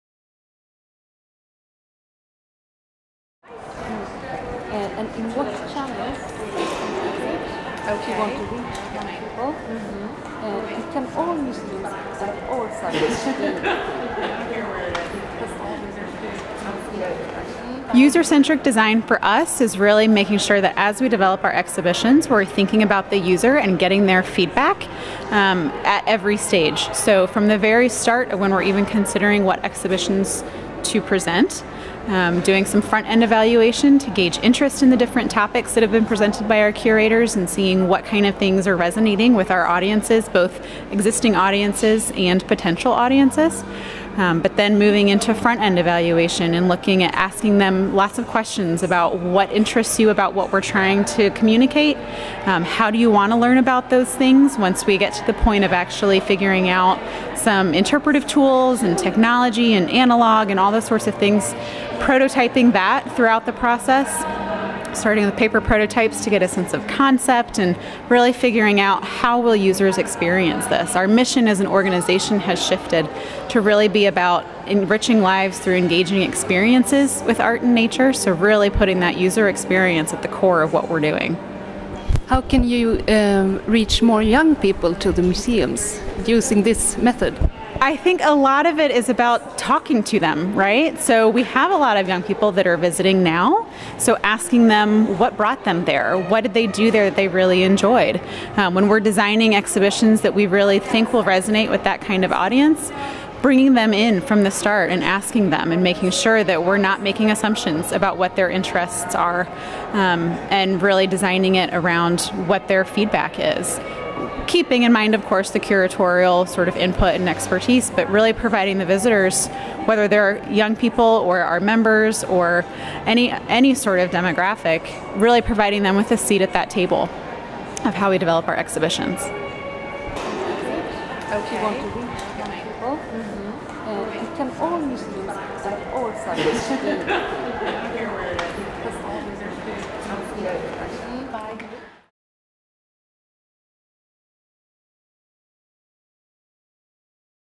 Emily Hellmuth, Indianapolis Museum of Art, om user-centric design
Emily Hellmuth, Manager of Interpretation på Indianapolis Museum of Art intervjuas om bland annat user-centric design på konferensen Museums and the Web 2016 i Los Angeles.